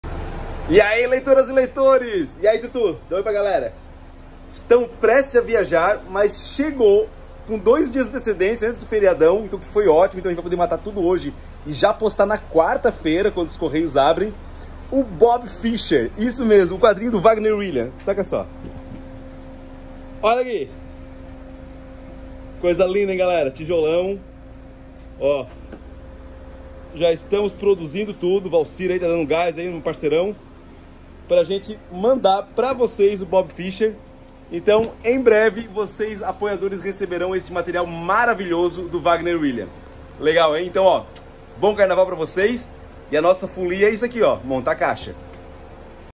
BOBBY FISCHER está entre nós!
BOBBY FISCHER - UMA VIDA EM PRETO DE BRANCO chegou! partiu ler Julian Voloj e Wagner Willian. Envio nesta sexta aos apoiadores. Quem perdeu a campanha, já está na amazon (envio final de março).
Tradução de Dandara Palankof.